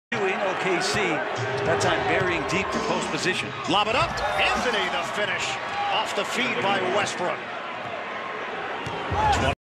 NBA, playoff 2018, Thunder vs. Jazz, Round 1, Game 4, Move 12, Russell Westbrook, assist
Oklahoma City Thunder - Utah Jazz, Game 4, 23/04/2018, quarter 1
Russell Westbrook, assist